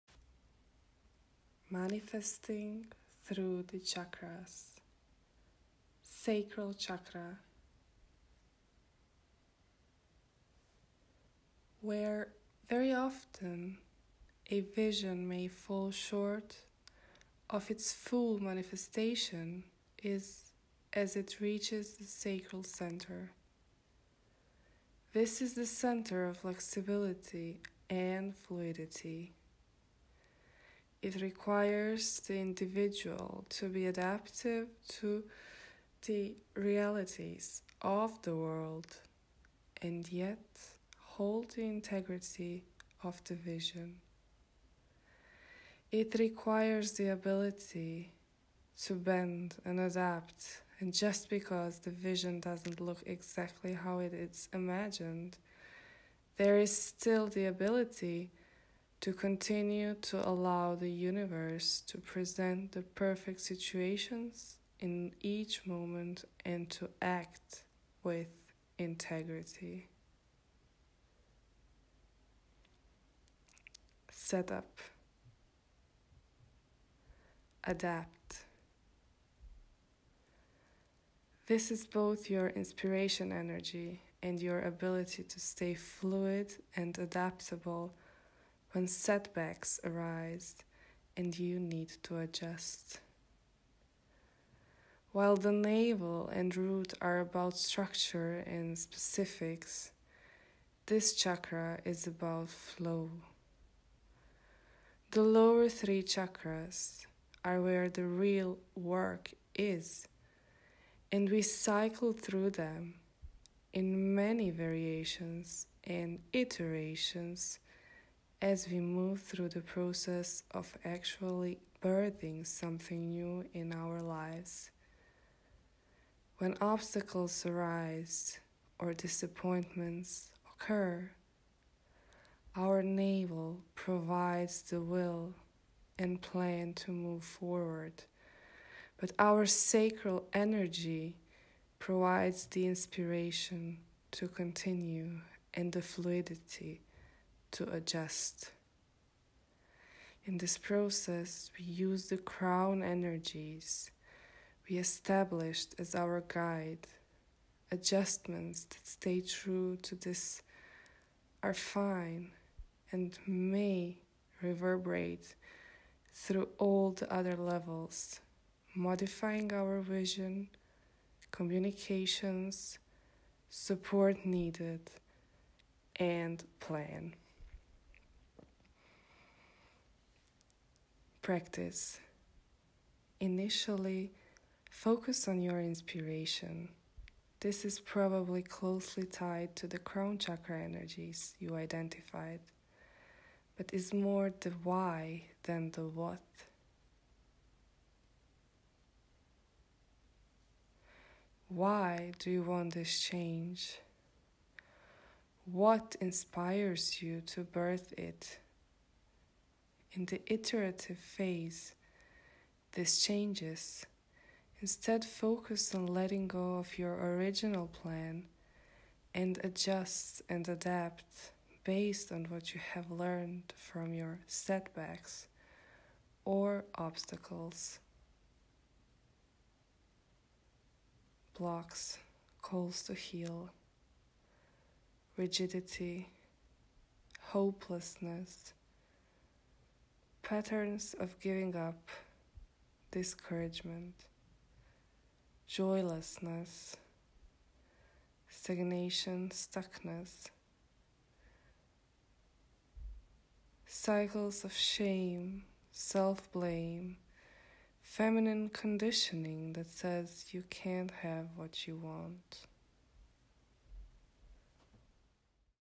Manifesting through the Chakras: Sacral Chakra (Svadhishtana)
A Guided meditation by the House of Prosperity (Golden Shield Institute, Hamburg)